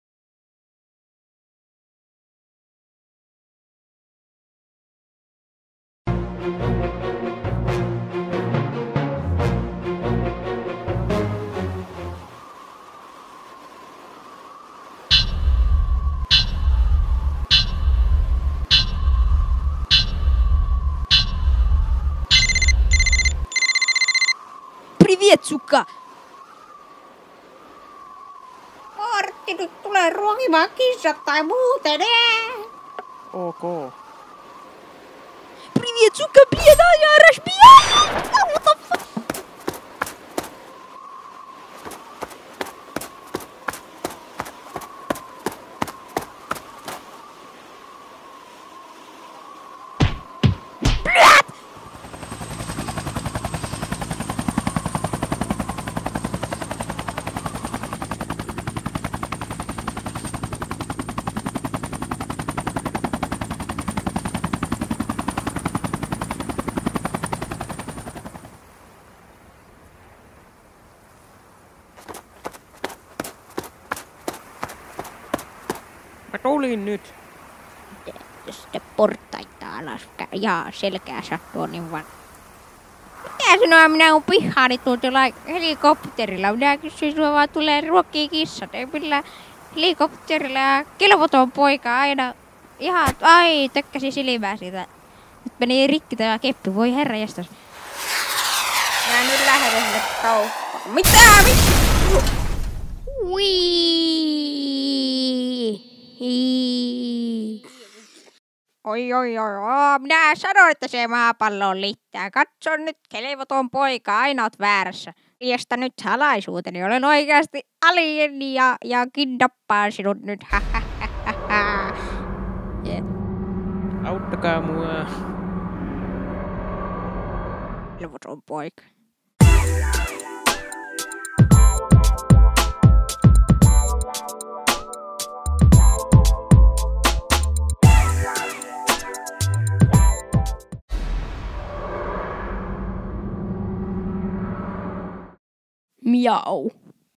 Martti 2 - The great escape, Animaatioseikkailu Animoi, Kesä 2019
Kesällä 2019 animaatioleirillä tehty animaatio.